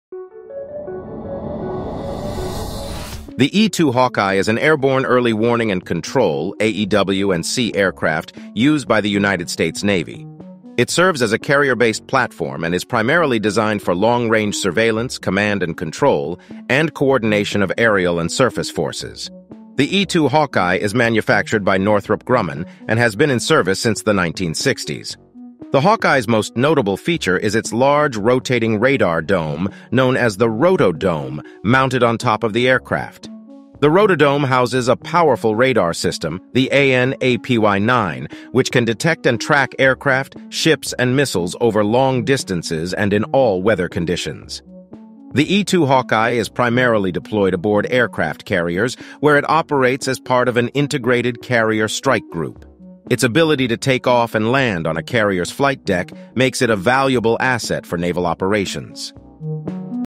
The aircraft that gives superb vision to its legion
The E-2 Hawkeye is an airborne early warning and control (AEW&C) aircraft used by the United States Navy. It serves as a carrier-based platform and is primarily designed for long-range surveillance, command and control, and coordination of aerial and surface forces. The E-2 Hawkeye is manufactured by Northrop Grumman and has been in service since the 1960s.
The Hawkeye's most notable feature is its large rotating radar dome, known as the "rotodome," mounted on top of the aircraft. The rotodome houses a powerful radar system, the AN/APY-9, which can detect and track aircraft, ships, and missiles over long distances and in all weather conditions. The E-2 Hawkeye is primarily deployed aboard aircraft carriers, where it operates as part of an integrated carrier strike group. Its ability to take off and land on a carrier's flight deck makes it a valuable asset for naval operations.